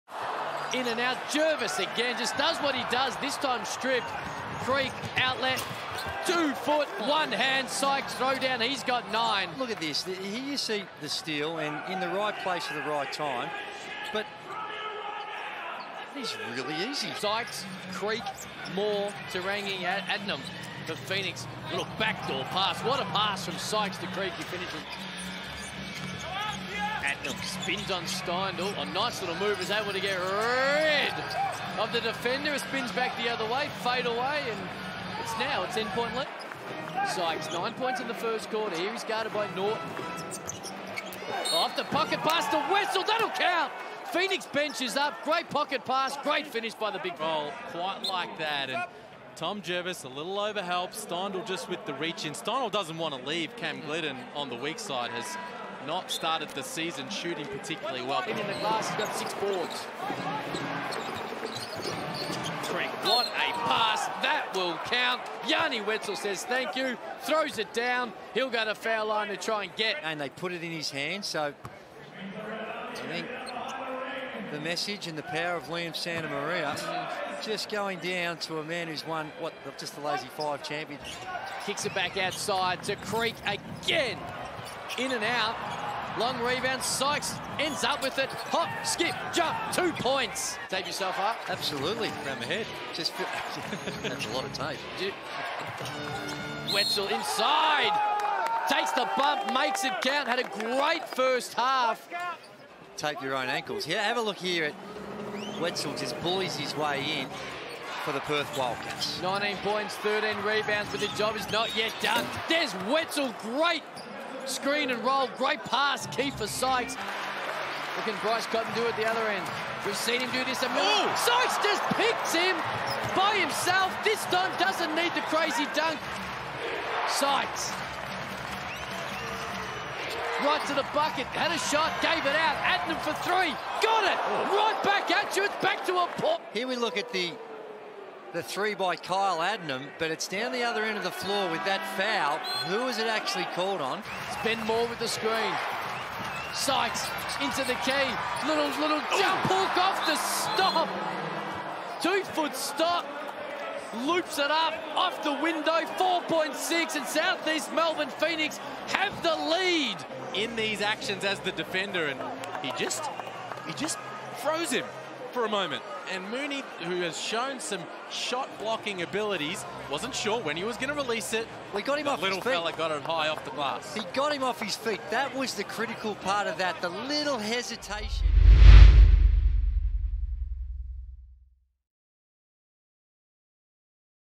Match highlights vs Perth Wildcats (Round 3, NBL21)
All the best highlights from our 1 point win against the Perth Wildcats at RAC Arena in Round 3 of NBL21.